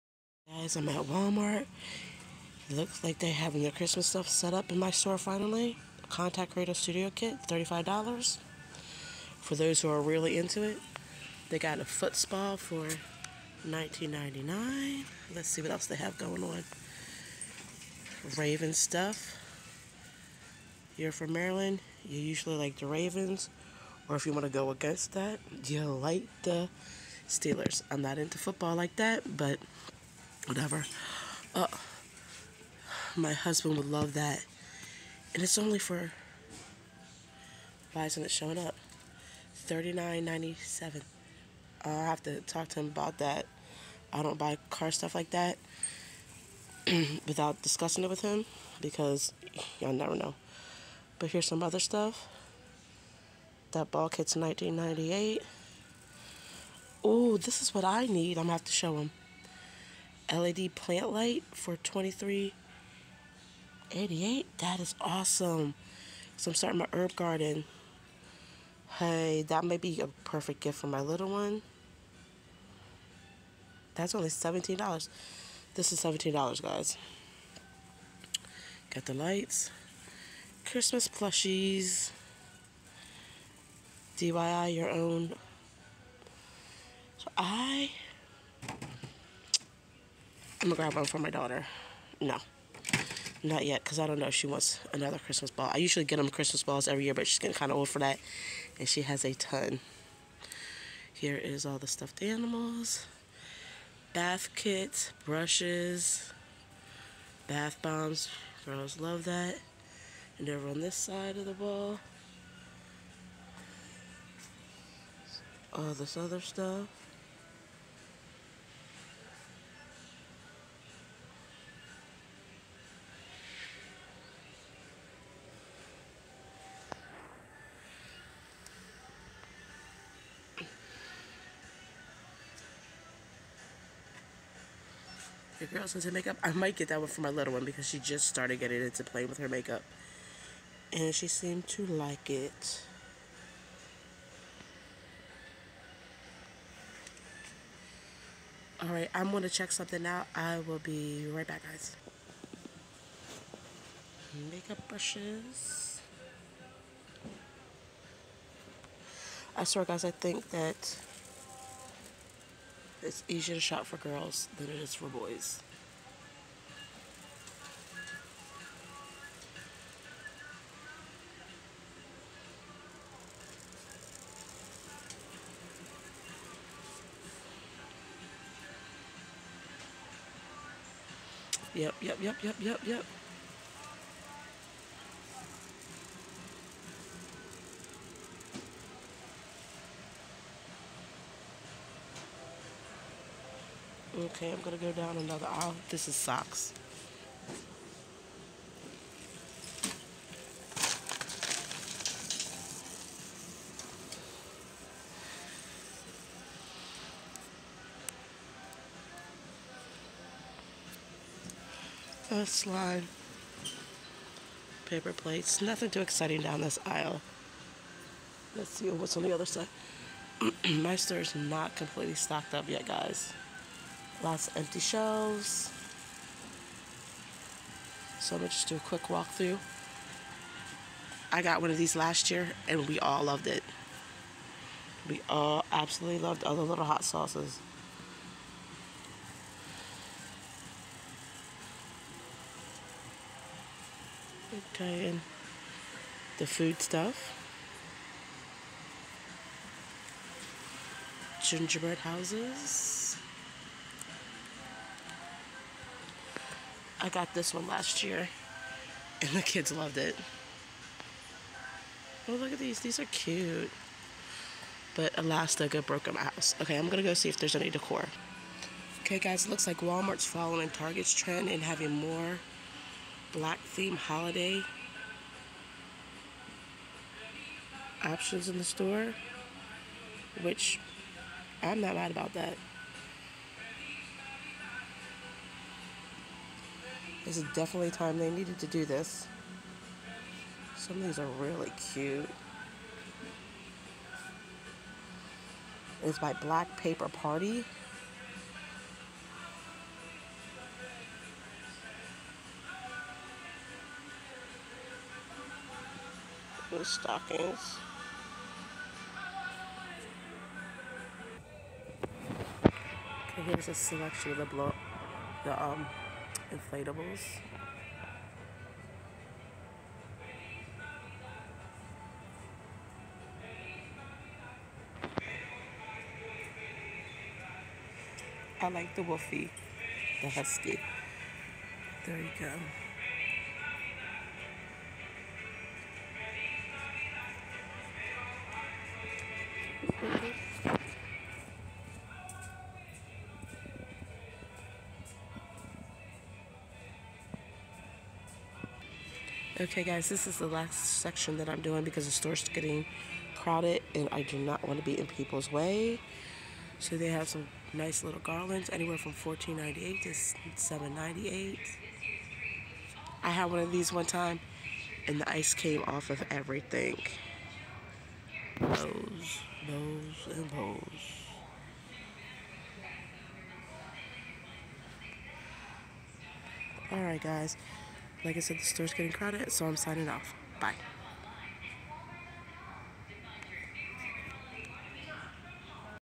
Walmart Christmas walk thru
ran to Walmart to see what was put out for Christmas